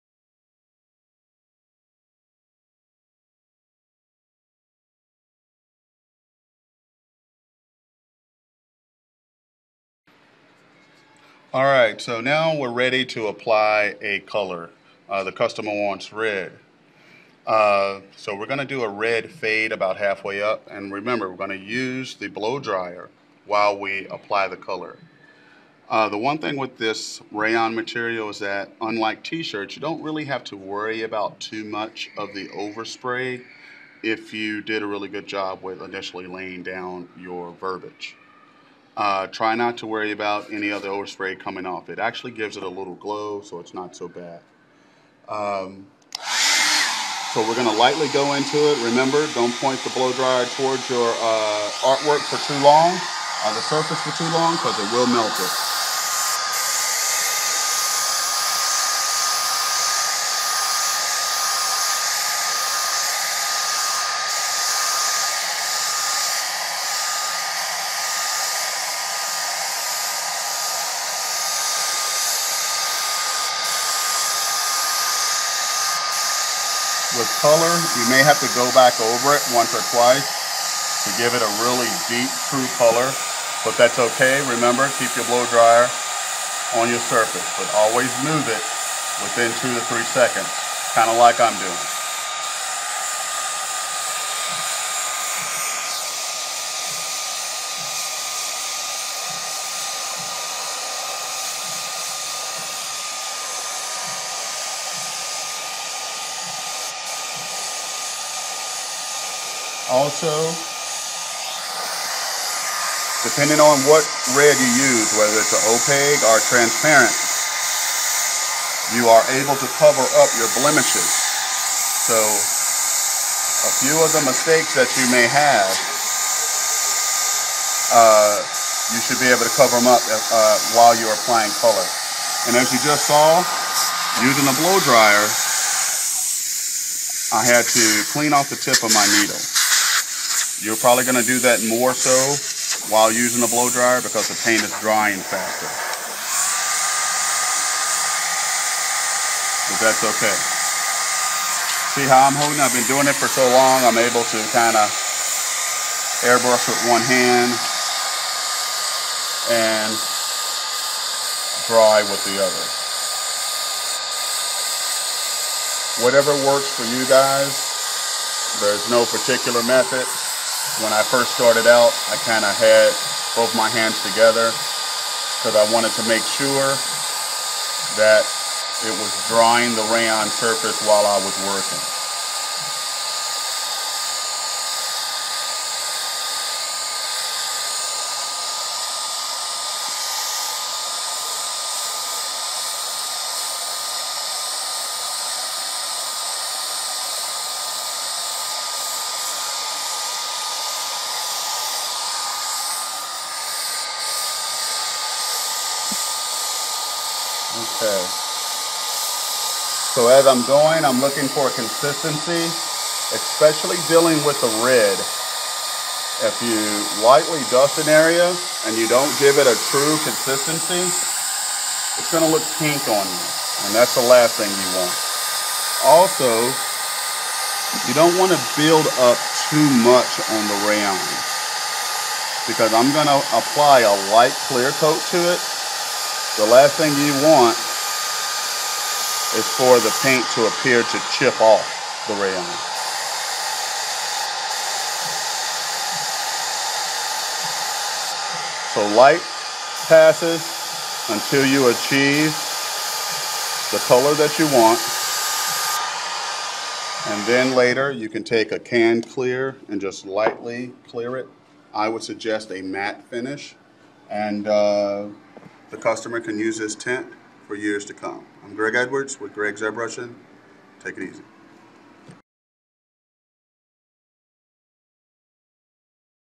How to Airbrush Letters on Rayon Part 2
This is a basic tutorial on airbrushing letters on rayon material, in this case a tent cover.